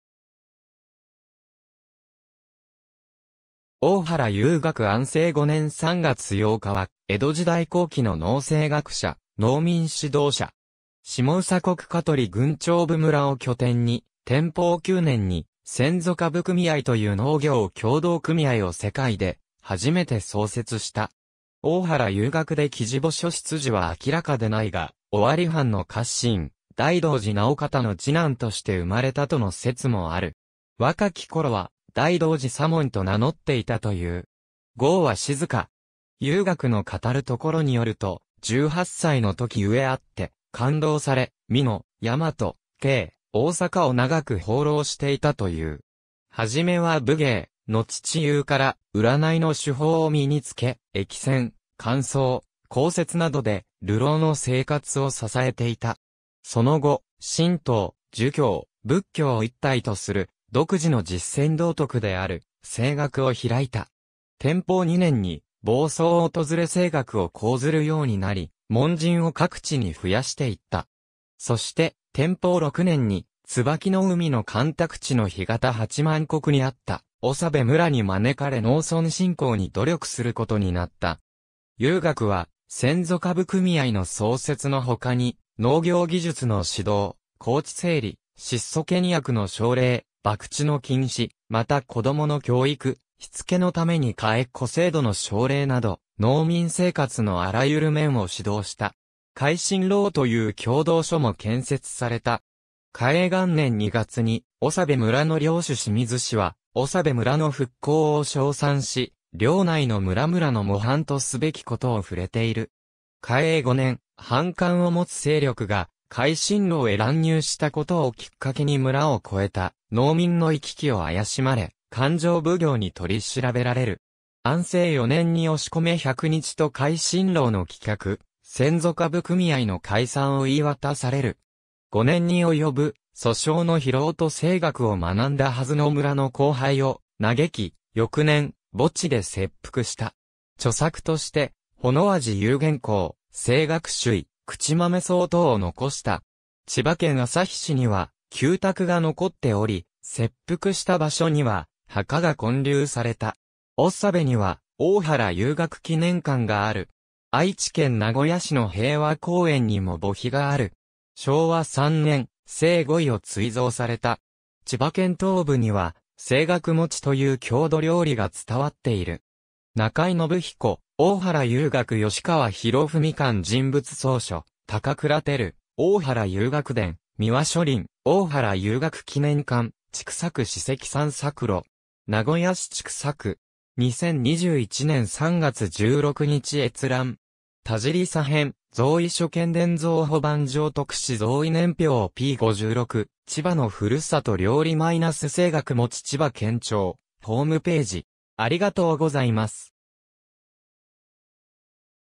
大原幽学
大原 幽学（おおはら ゆうがく、寛政9年3月17日（1797年4月13日） - 安政5年3月8日（1858年4月21日））は、江戸時代後期の農政学者、農民指導者。
下総国香取郡長部（ながべ）村（現在の旭市長部）を拠点に、天保9年（1838年）に先祖株組合という農業協同組合を世界で初めて創設した。
大原幽学出生地（愛知県名古屋市中区の愛知県図書館） 墓所（愛知県名古屋市千種区の平和公園） 出自は明らかでないが、尾張藩の家臣大道寺直方の次男として生まれたとの説もある。
若き頃は大道寺左門と名乗っていたという。
号は静香。
幽学の語るところによると、18歳のとき故あって勘当され、美濃、大和、京、大阪を長く放浪していたという。
初めは武芸、のち知友から占いの手法を身に付け、易占、観相、講説などで流浪の生活を支えていた。
その後、神道、儒教、仏教を一体とする、独自の実践道徳である「性学」（「生理学」「性理道」ともいった）を開いた。
天保2年（1831年）に房総を訪れ性学を講ずるようになり、門人を各地に増やしていった。
そして、天保6年（1835年）に椿海の干拓地の干潟八万石にあった長部村に招かれ農村振興に努力することになった。
幽学は、先祖株組合の創設のほかに、農業技術の指導、耕地整理、質素倹約の奨励、博打の禁止、また子供の教育・しつけのために換え子制度の奨励など、農民生活のあらゆる面を指導した。
「改心楼」という教導所も建設された。
嘉永元年（1848年）2月に、長部村の領主清水氏は、長部村の復興を賞賛し、領内の村々の模範とすべきことを触れている。
嘉永5年（1852年）、反感を持つ勢力が改心楼へ乱入したことをきっかけに村を越えた農民の行き来を怪しまれ、勘定奉行に取り調べられる。
安政4年（1857年）に押込百日と改心楼の棄却、先祖株組合の解散を言い渡される。
5年に及ぶ訴訟の疲労と性学を学んだはずの村の荒廃を嘆き、翌年、墓地で切腹した。
著作として『微味幽玄考』『性学趣意』『口まめ草』等を遺した。
千葉県旭市には旧宅（国の史跡）が残っており、切腹した場所には墓が建立された。
長部には「大原幽学記念館」がある。
愛知県名古屋市の平和公園（公園内の萬松寺墓域）にも墓碑がある。
昭和3年（1928年）、正五位を追贈された。
千葉県東部には、「性学もち」という郷土料理が伝わっている。
^ 中井信彦『大原幽学』吉川弘文館人物叢書（1989年5月） ^ 高倉テル『大原幽学伝』美和書林（1949年） ^ 大原幽学記念館（2020年4月12日閲覧） ^ “千種区史跡散策路”. 名古屋市千種区. 2021年3月16日閲覧。
^ 田尻佐 編『贈位諸賢伝 増補版 上』（近藤出版社、1975年）特旨贈位年表 p.56 ^ ちばのふるさと料理 - 性学もち千葉県庁ホームページ（2020年4月12日閲覧）